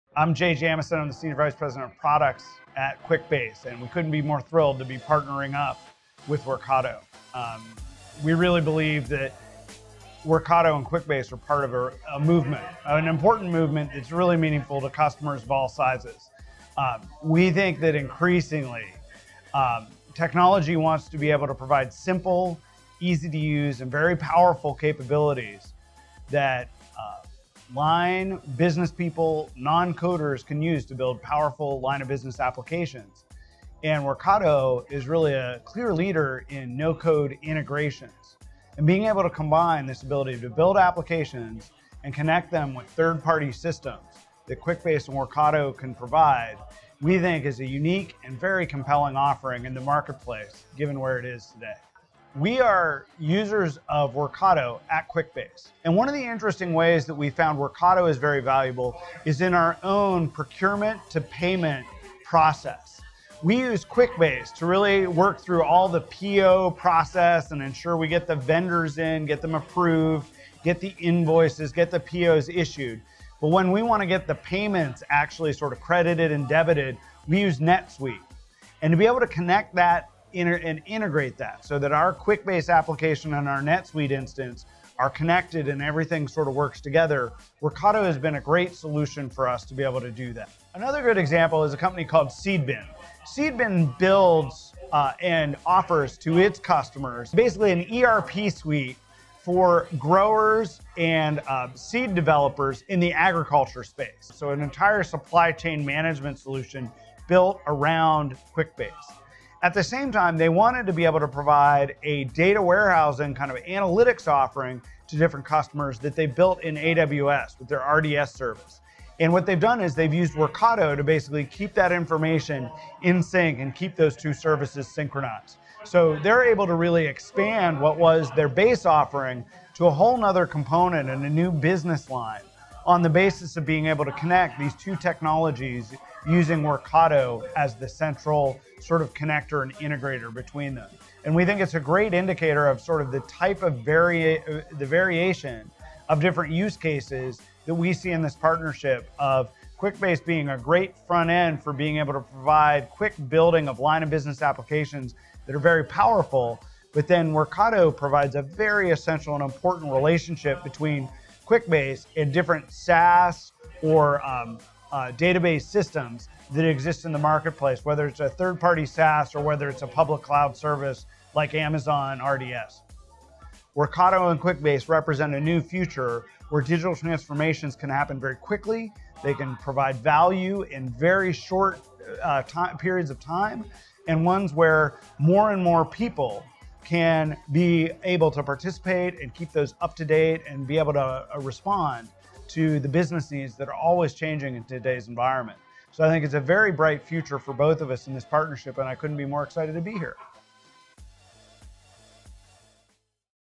Quick Base & Workato | Customer Stories
Jay Jamison, SVP Products of Quick Base discusses the partnership with Workato, their internal use of Workato, and use cases being enabled by Quick Base and Workato.

"A new future where digital transformations can happen very quickly and provide value in very short periods of time".